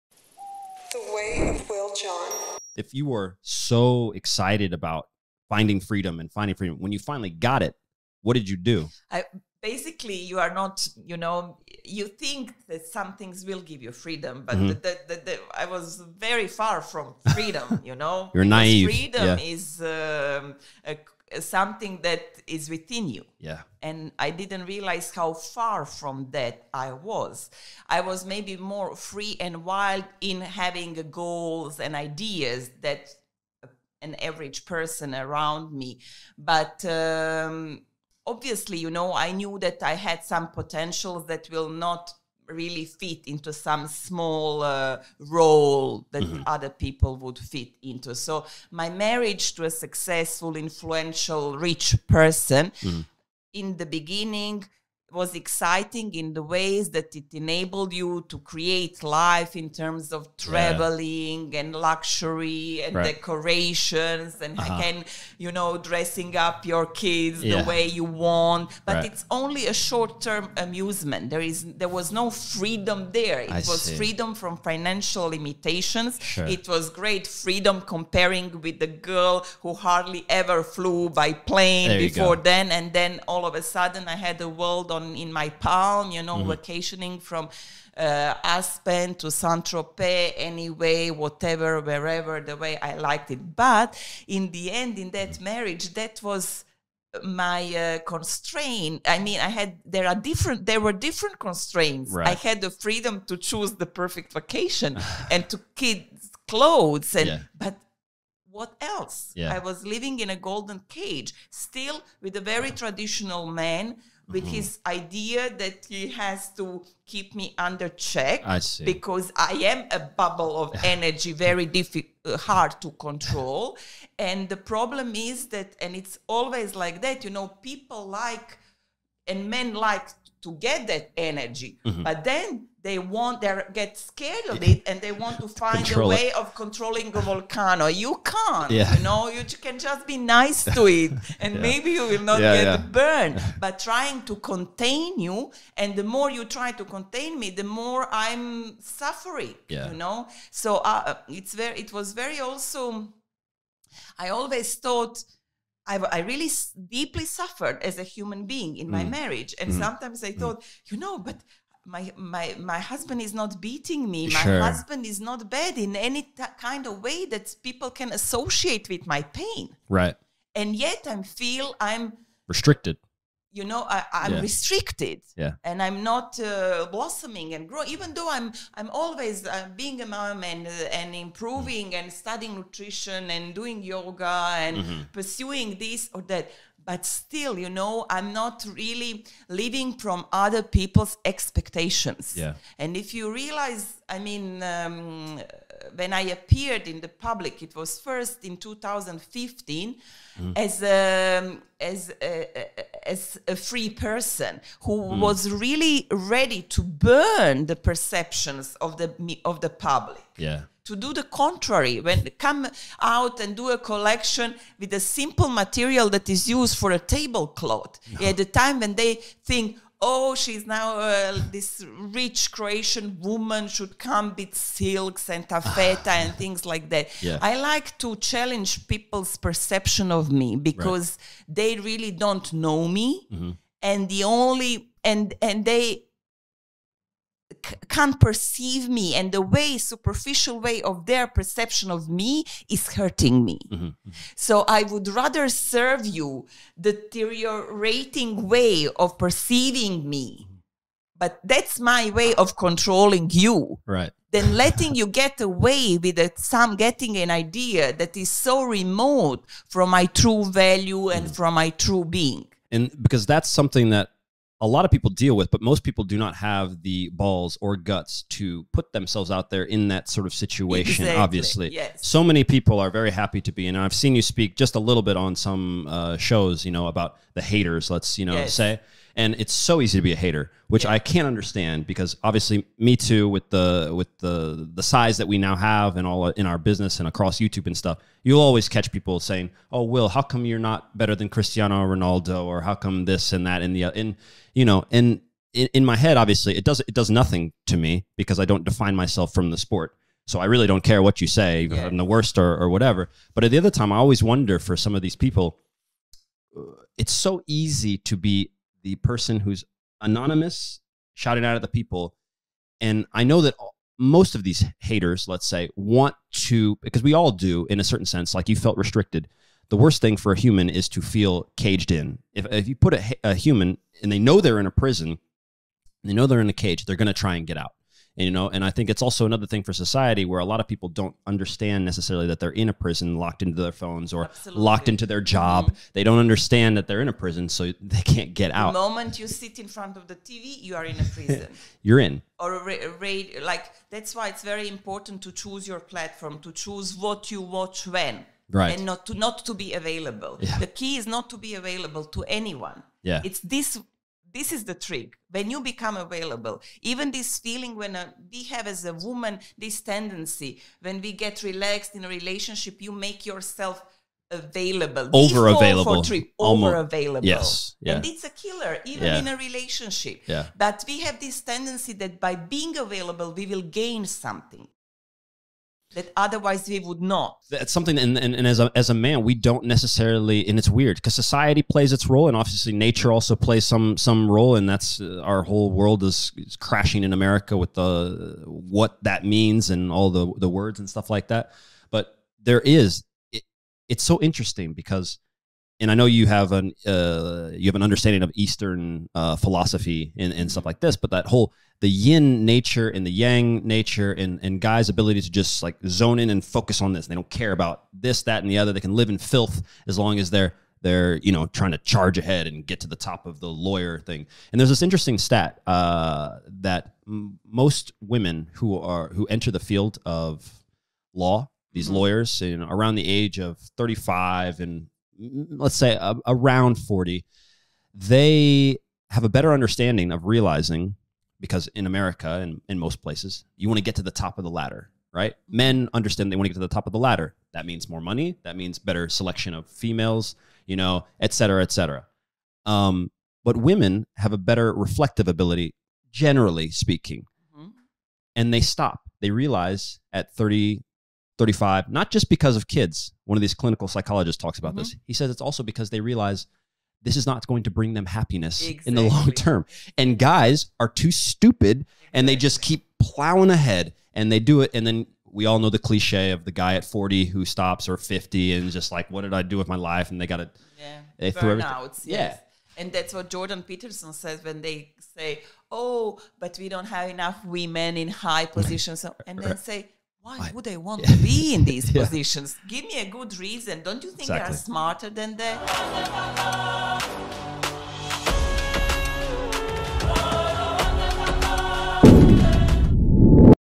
What it's REALLY like to be INSANELY RICH | Borna Kotromanic
👤 Borna Kotromanic is well-known for her highly publicized divorce from one of Croatia’s richest men, but she is so much more than a frivolous celebrity. An intellectual, a philosopher with an unusual life trajectory, she selflessly shares her insights on social media platforms with a playful but pungent delivery, providing entertainment for the Croatian public underscored by profound wisdom for those who want to hear it.